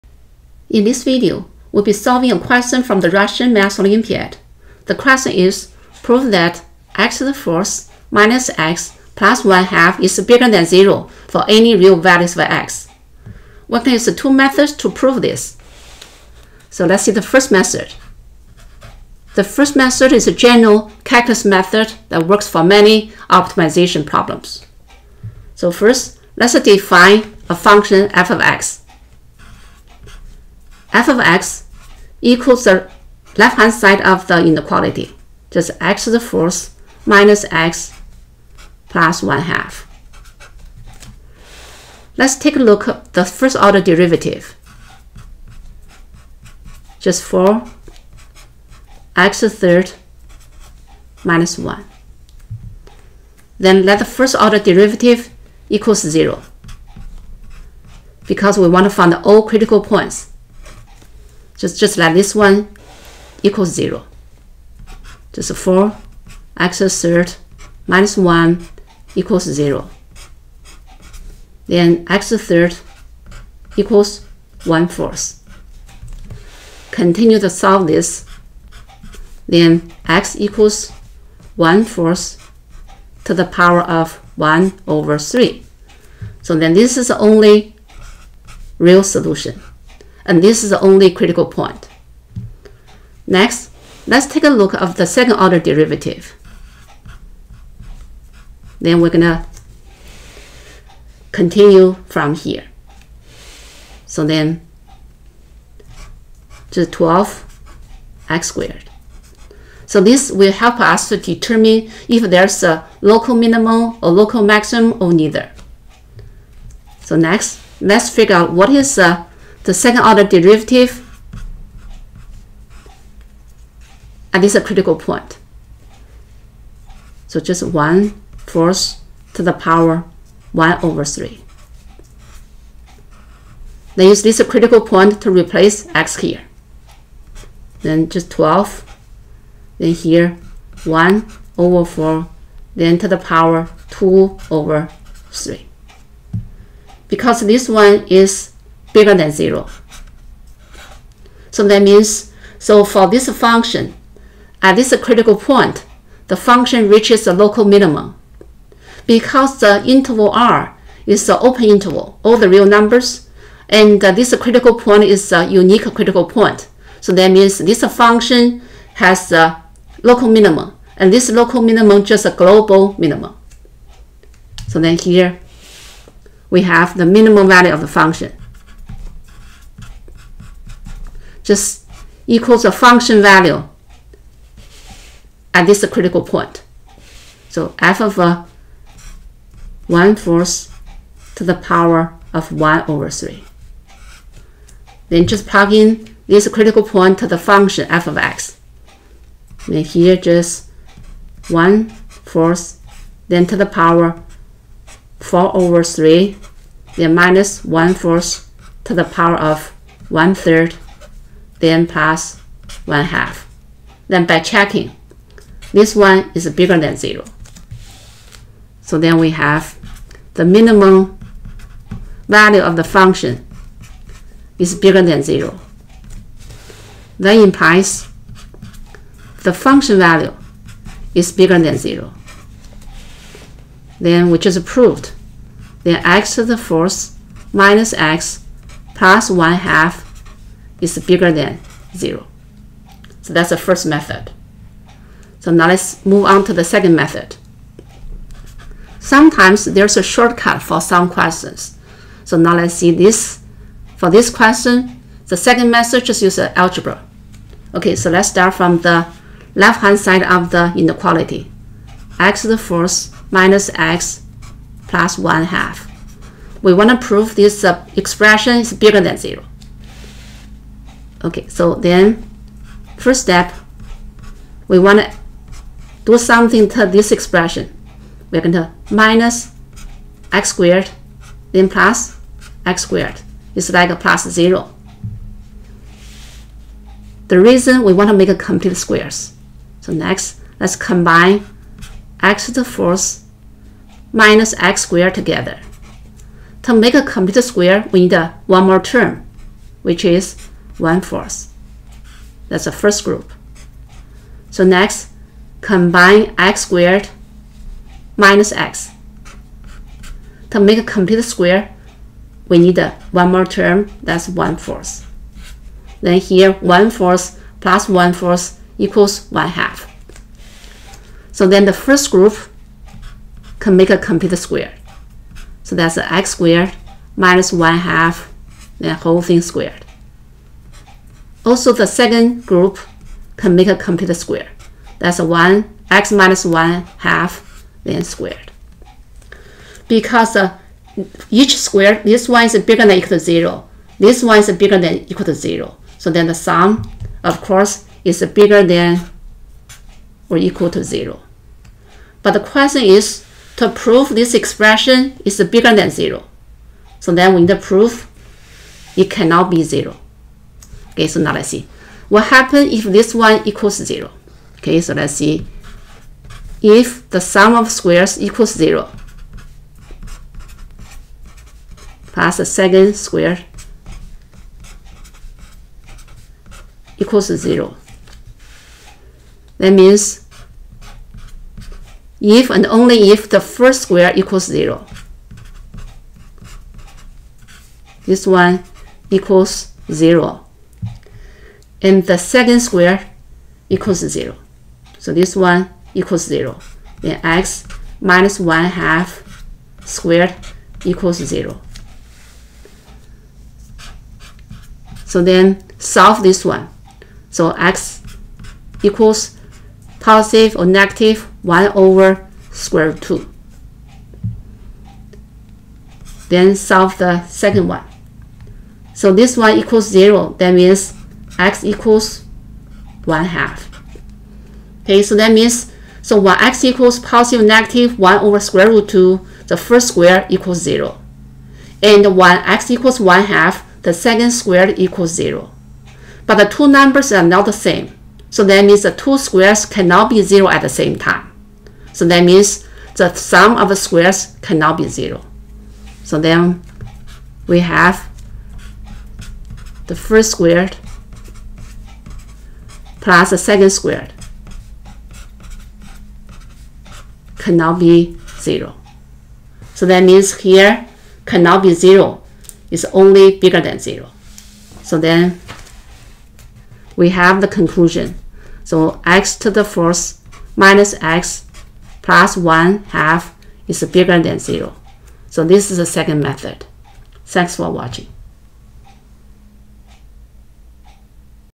Math Olympiad Question | Inequality | Russian Math Olympiad
How to solve this Math Olympiad question?  This video presents how to use cauculus and algebra techniques and tricks to prove this inequality quickly and easily. This question is from the Russian Math Olympiad. For another challenge Math Olympiad question